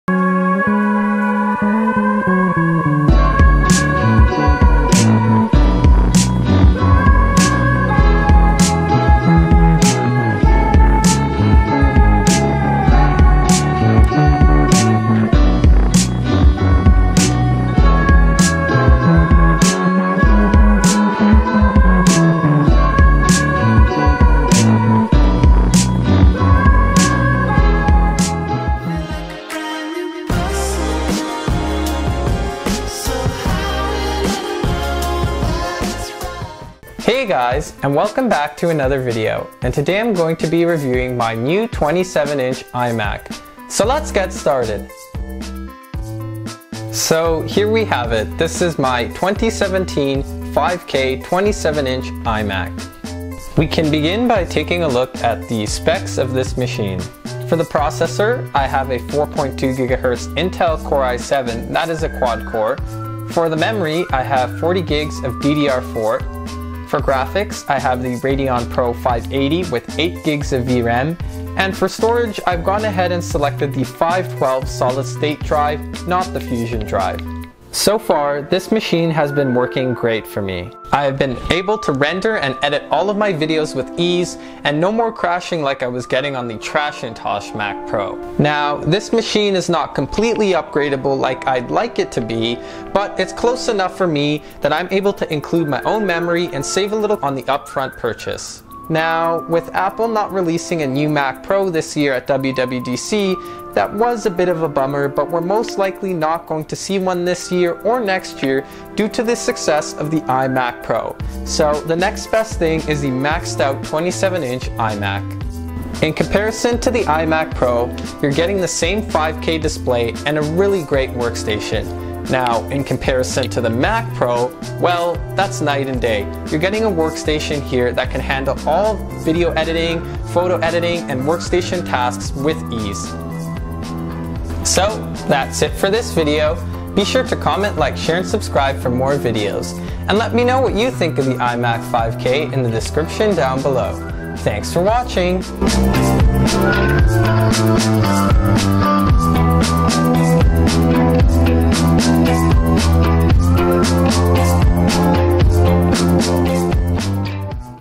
iMac 27” 5K Review 2018!
With so many Apple computers on the market what is the best iMac for video editing?

Is the iMac 5K 27" right for you?